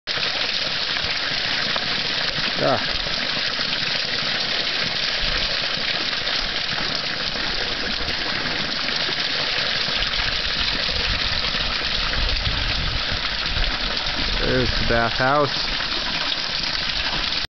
Summer Lake H S outside hot pool
Just outside to the west of the historic bath house the owners have built an exterior soaking pool. It still needs some work and further development but the pool allows a soaker to appreciate the beautiful scenery and/or magnificent nighttime star show.